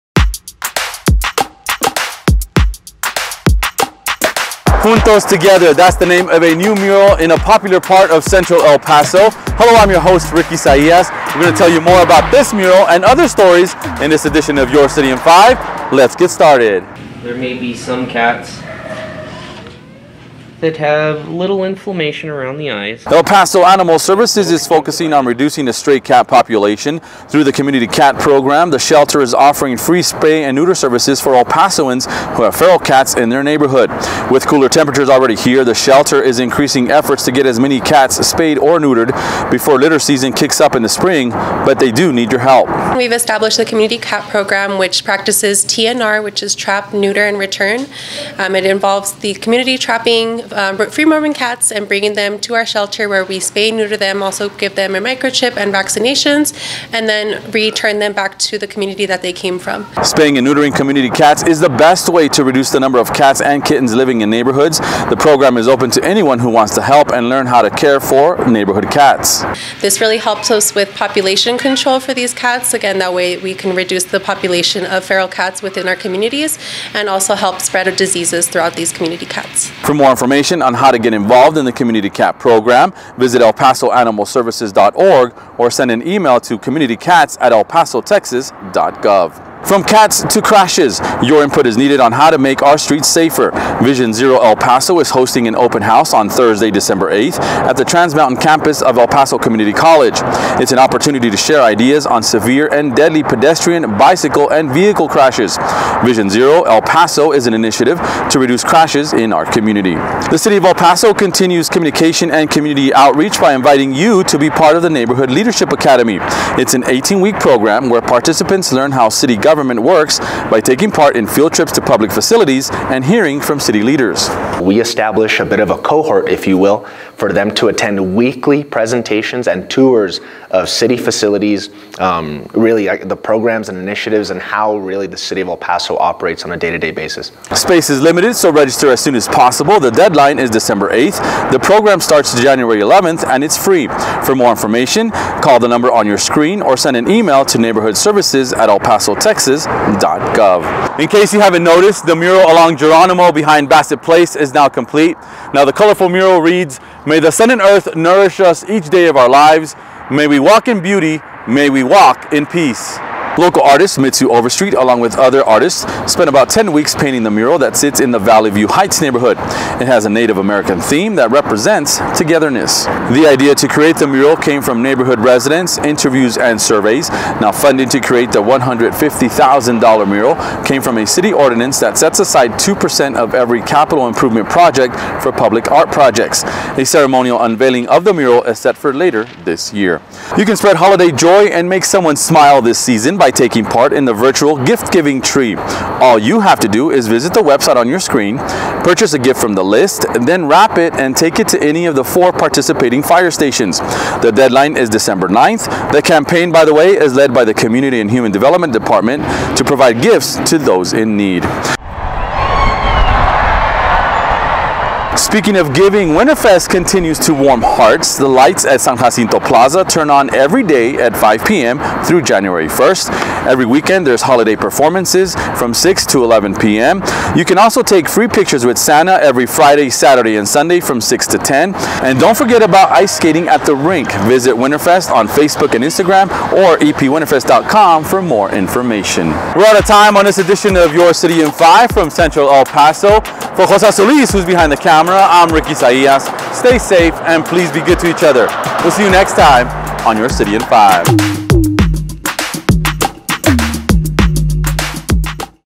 YC5 - New Art is the Cat's Meow!
This YC5 is packed with valuable information! Check it out El Paso!
Controlling Feral Cat Population
Input Needed on Crashes
Learn About City Government
Central El Paso Mural Complete
Virtual Gift Giving Tree
WinterFest Weekend Activities